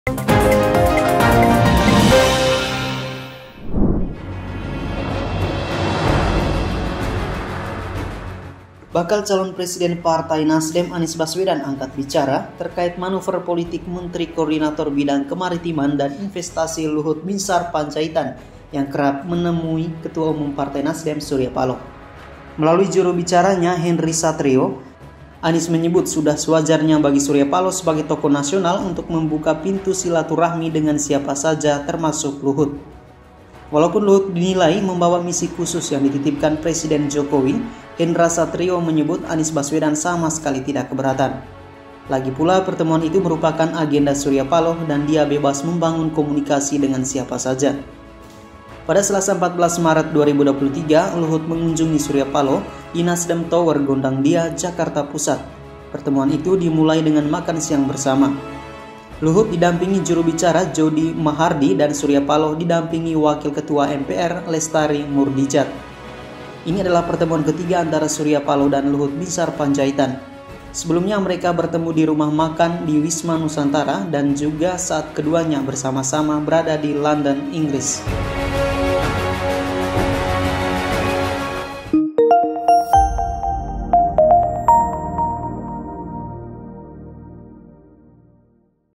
Anies Beri Respon Santai Saat Surya Paloh Bertemu Dengan Luhut
Bakal calon presiden Partai NasDem Anies Baswedan angkat bicara terkait manuver politik Menteri Koordinator Bidang Kemaritiman dan Investasi Luhut Binsar Pandjaitan yang kerap menemui Ketua Umum NasDem Surya Paloh.
Melalui juru bicaranya Hendro Satrio, Anies menyebut sudah sewajarnya bagi Surya Paloh sebagai tokoh nasional untuk membuka pintu silaturahmi dengan siapa saja, termasuk Luhut.
Walaupun Luhut dinilai membawa misi khusus yang dititipkan Presiden Jokowi, Hendra Satrio menyebut Anies Baswedan sama sekali tidak keberatan. Lagi pula, pertemuan itu merupakan agenda Surya Paloh dan dia bebas membangun komunikasi dengan siapa saja.
Pada Selasa 14 Maret 2023, Luhut mengunjungi Surya Paloh di NasDem Tower, Gondangdia, Jakarta Pusat. Pertemuan itu dimulai dengan makan siang bersama. Luhut didampingi Juru bicara Jodi Mahardi dan Surya Paloh didampingi Wakil Ketua MPR Lestari Moerdijat.
 Ini adalah pertemuan ketiga antara Surya Paloh dan Luhut Binsar Pandjaitan. Sebelumnya mereka bertemu di rumah makan di Wisma Nusantara dan juga saat keduanya sama-sama berada di London, Inggris.